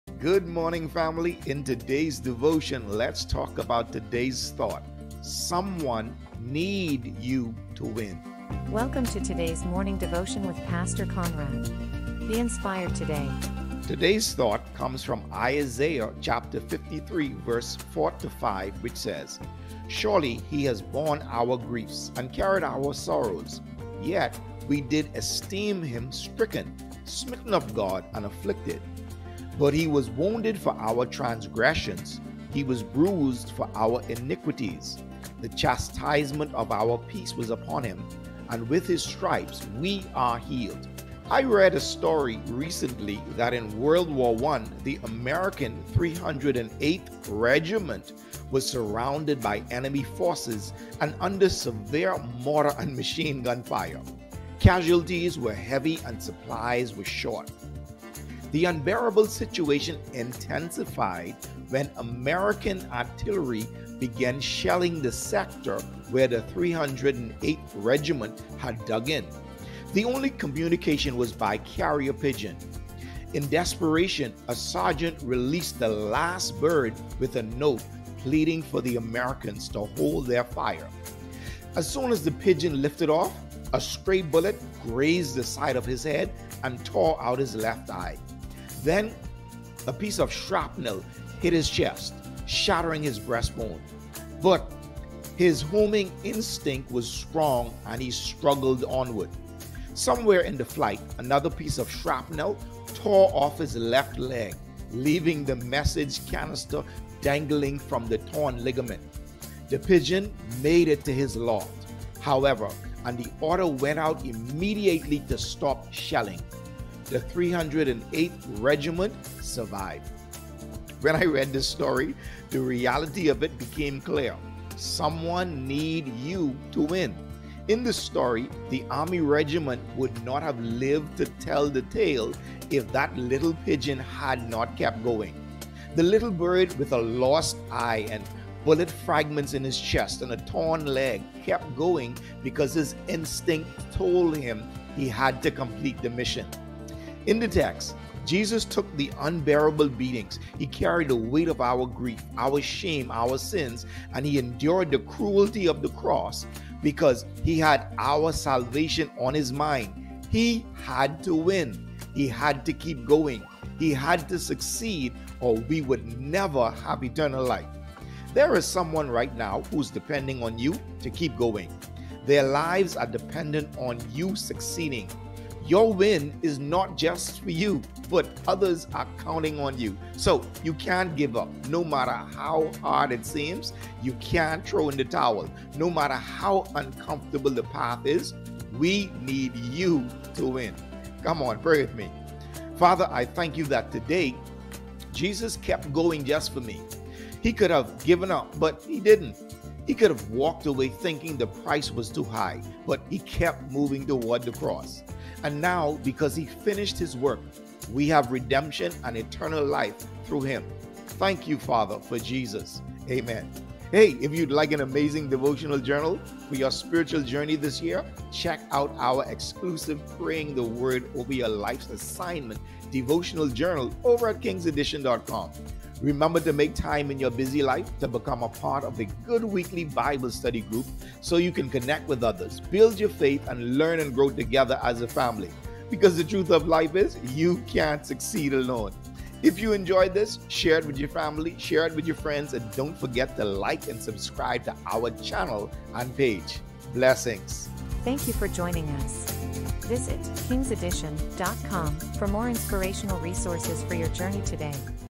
Someone Need You To Win | Conrad L Jones
In today’s devotion, let’s talk about today's thought, “Someone Need You To Win”.

SCRIPTURE | Today's thought comes from Isaiah 53:4-5 (KJV) which says, “4 Surely he hath borne our griefs, and carried our sorrows: yet we did esteem him stricken, smitten of God, and afflicted. 5 But he was wounded for our transgressions, he was bruised for our iniquities: the chastisement of our peace was upon him; and with his stripes we are healed.”

👉Get your copy of the devotional journal "Praying The Word Over Your Life's Assignment": kttps://kingsaddition.com